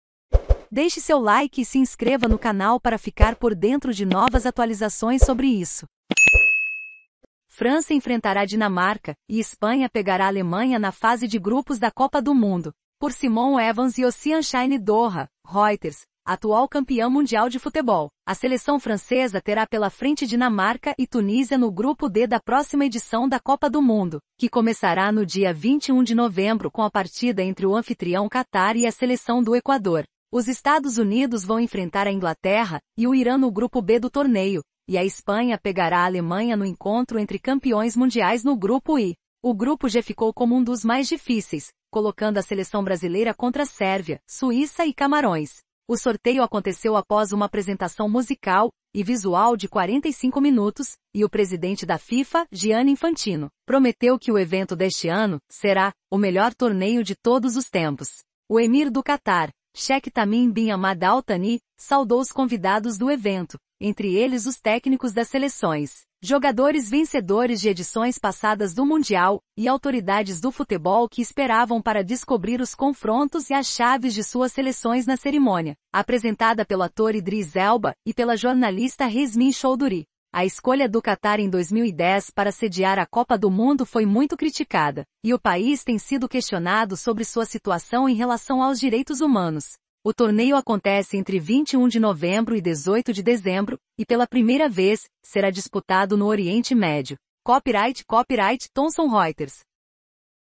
FRANÇA ENFRENTARÁ DINAMARCA E ESPANHA PEGARÁ ALEMANHA NA FASE DE GRUPOS DA COPA DO...
França enfrentará Dinamarca e Espanha pegará Alemanha na fase de grupos da Copa do Mundo

Por Simon Evans e Ossian Shine DOHA (Reuters)   Atual campeã mundial de futebol. a seleção francesa terá pela frente Dinamarca e Tunísia no Grupo D da próxima edição da Copa do Mundo. que começará no dia 21 de novembro com a partida entre o anfitrião Catar e a seleção do Equador. Os Estados Unidos vão enfrentar a Inglaterra e o Irã no Grupo B do torneio. e a Espanha pegará a Alemanha no encontro entre campeões mundiais no Grupo E. O Grupo G ficou como um dos mais difíceis. colocando a seleção brasileira contra Sérvia. Suíça e Camarões. O sorteio aconteceu após uma apresentação musical e visual de 45 minutos. e o presidente da Fifa. Gianni Infantino. prometeu que o evento deste ano será 'o melhor torneio de todos os tempos'. O emir do Catar. xeque Tamim bin Hamad al Thani. saudou os convidados do evento. entre eles os técnicos das seleções. jogadores vencedores de edições passadas do Mundial e autoridades do futebol que esperavam para descobrir os confrontos e as chaves de suas seleções na cerimônia apresentada pelo ator Idris Elba e pela jornalista Reshmin Chowdhury. A escolha do Catar em 2010 para sediar a Copa do Mundo foi muito criticada. e o país tem sido questionado sobre sua situação em relação aos direitos humanos. O torneio acontece entre 21 de novembro e 18 de dezembro e pela primeira vez será disputado no Oriente Médio.